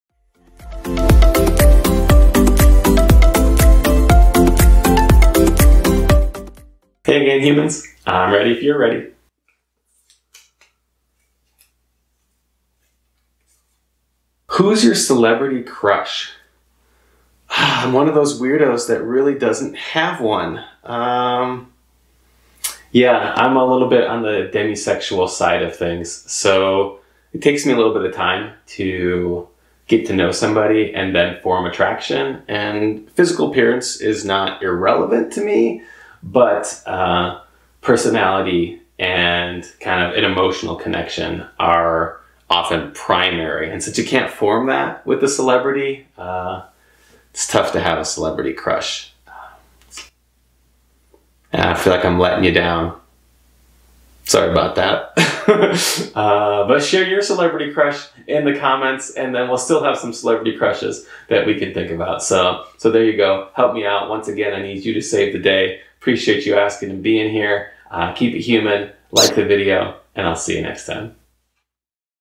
Who's your celebrity crush? Ask a Life Coach # 419
In this episode of Leveled Up's Ask A Life Coach, Mark Reinholz is asked who is his celebrity crush. He shares that he is, in part, demisexual so it takes time for him to get to know someone and develop attraction to them.